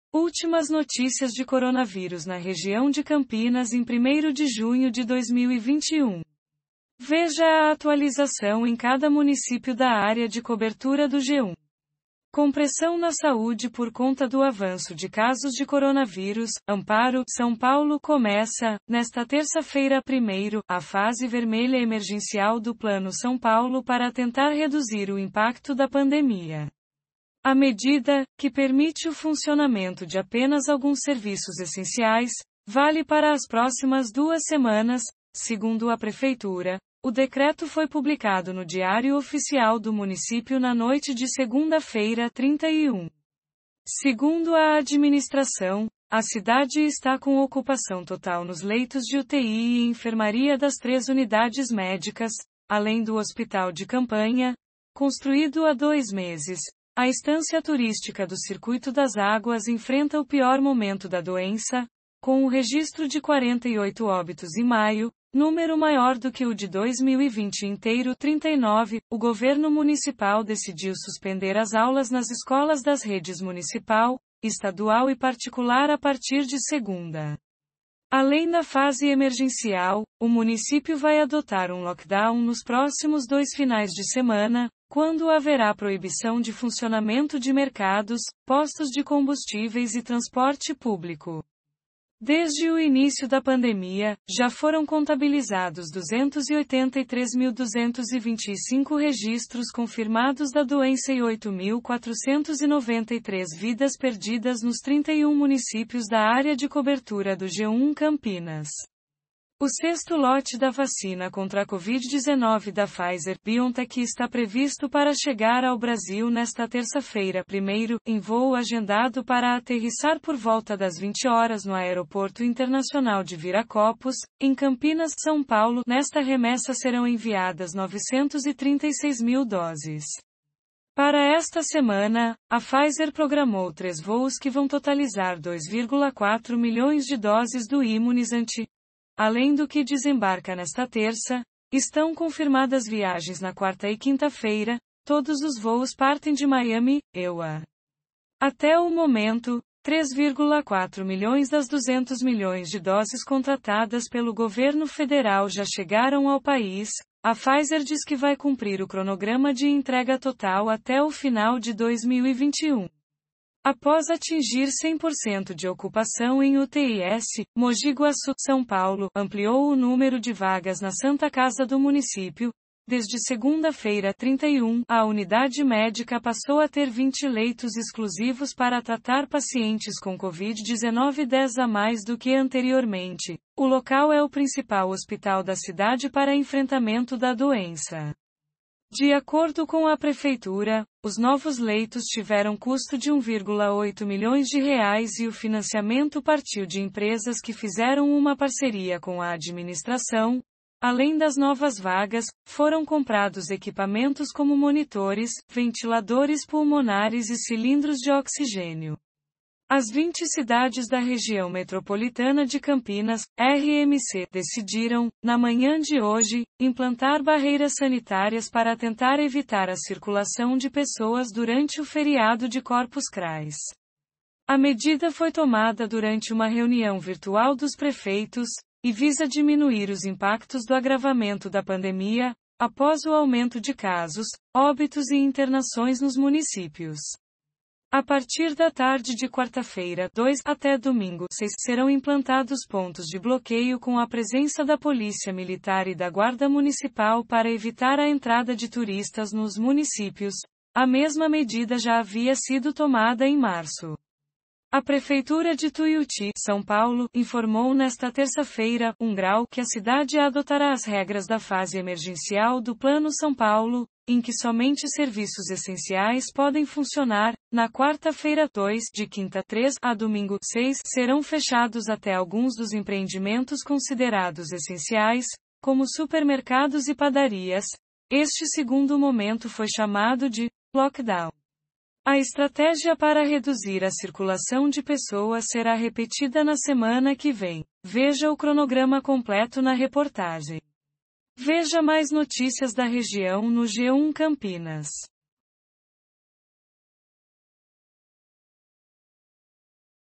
Últimas notícias de coronavírus na região de Campinas em 1º de junho de 2021
Veja a atualização em cada município da área de cobertura do G1.

Acessado em: 2 de junho de 2021 às 08:00